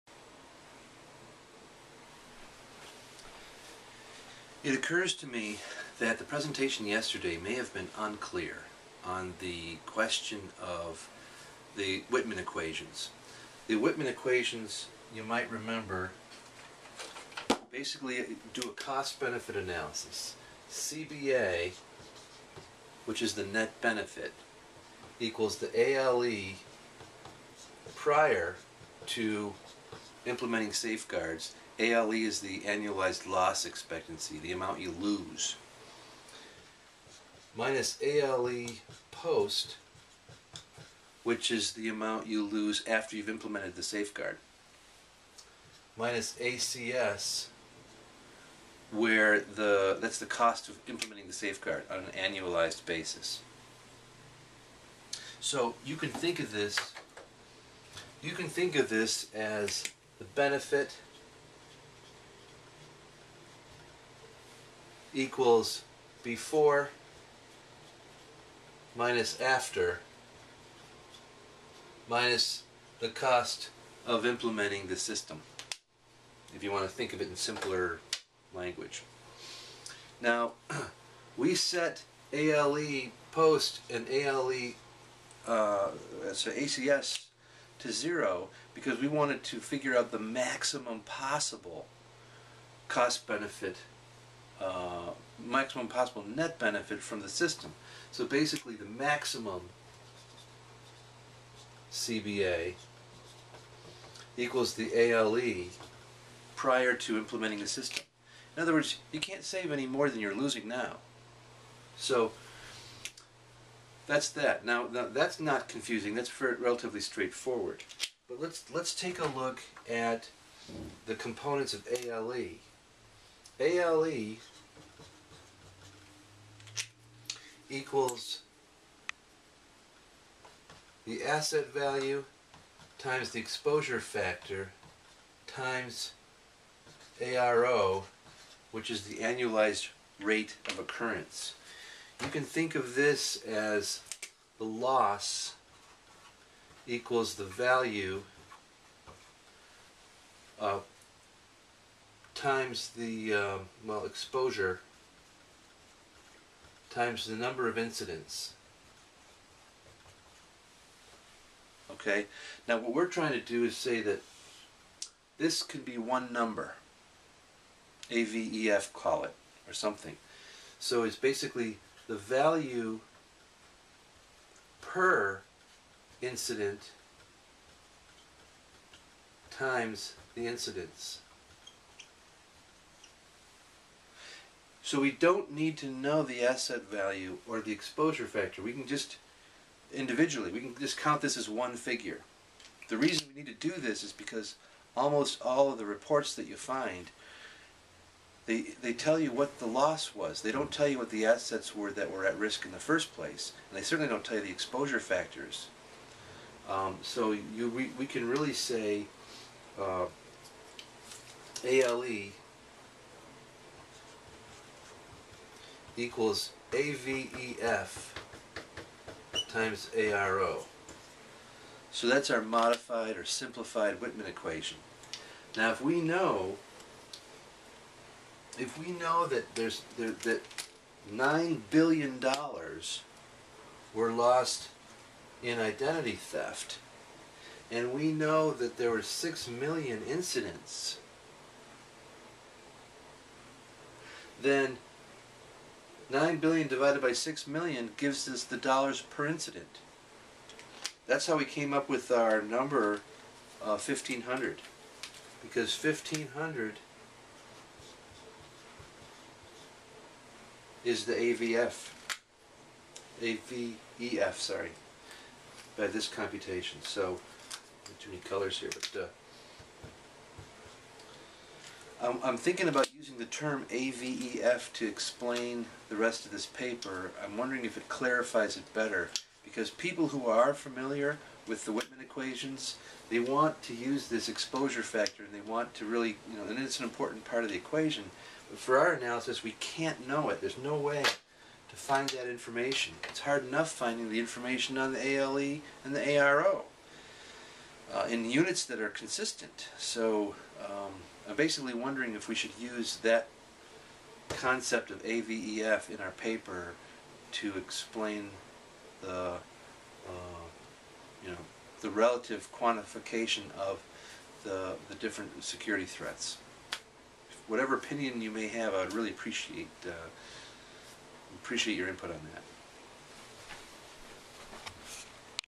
Information Security Equation Question: ALE = AVEF x
Dr Michael Whitman uses the equation ALE = AV x EF x ARO. Often the only information available is ALE and ARO. I propose using a new equation, ALE = AVEF x ARO, where AV and EF are combined into a single value. What is your opinion? Does this make analysis more clear?